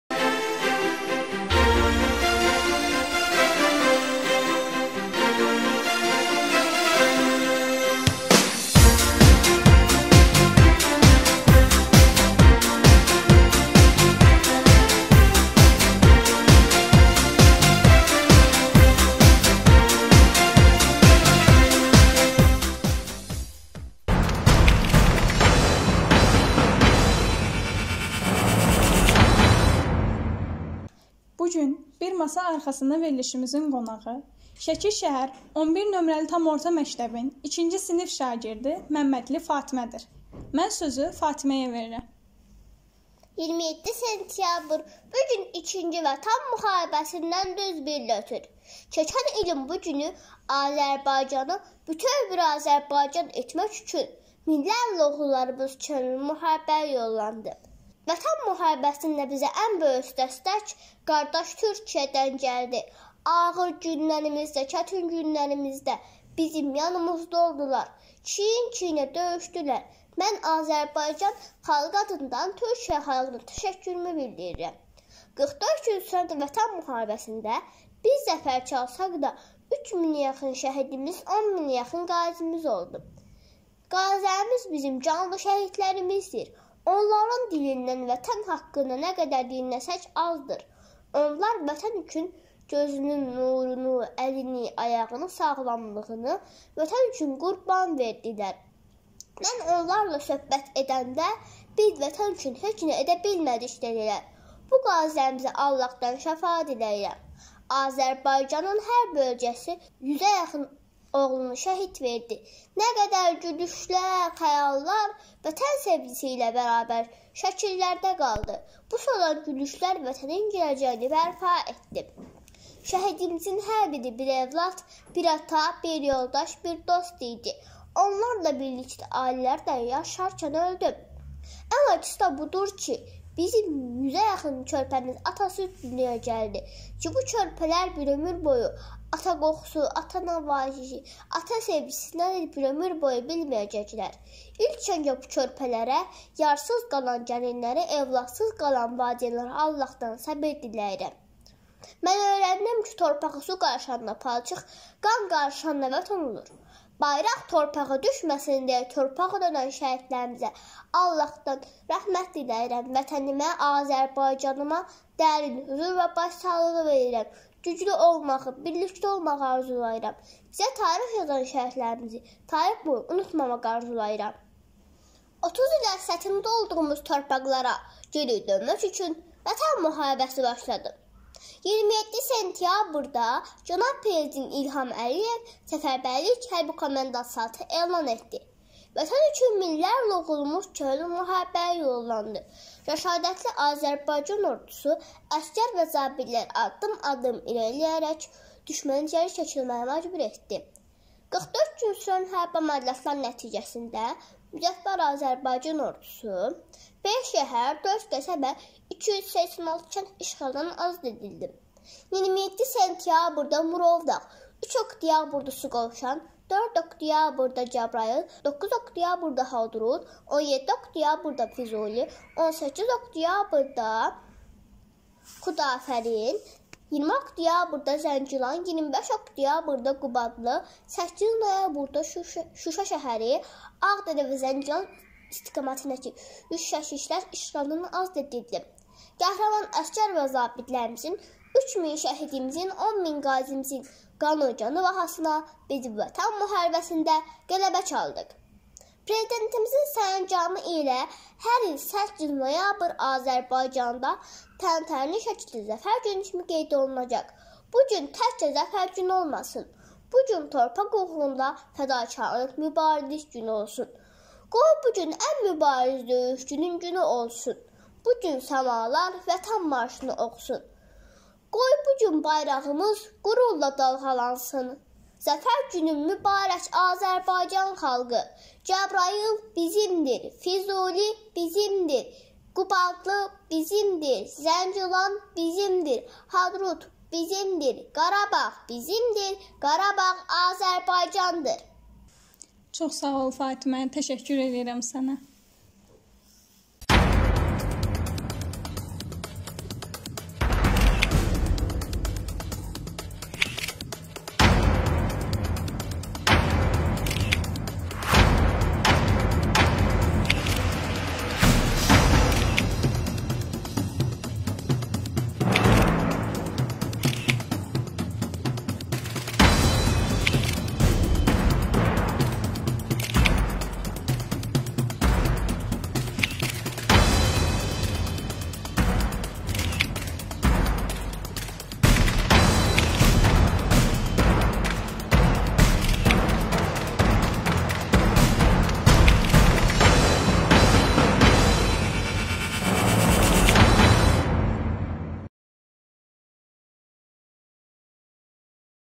Bir masa arxasında görüş verilişinin qonağı Məmmədli Fatimədir
BİR MASA ARXASINDA GÖRÜŞ
Azərbaycan Respublikasının Təhsil Nazirliyinin keçirdiyi Təhsildə İnkişaf və İnnovasiyalar üzrə  V Qrant müsabiqəsi layihəsinin qalibi
Rəşid bəy Əfəndiyev adına  Şəki şəhər 10 nömrəli tam orta məktəbin “Məktəb televiziyası” layihəsinin  - MTV10-nun studiyası
Verilişin qonağı Şəki şəhər 11 nömrəli tam orta məktəbin II sinif şagirdi Məmmədli Fatimədir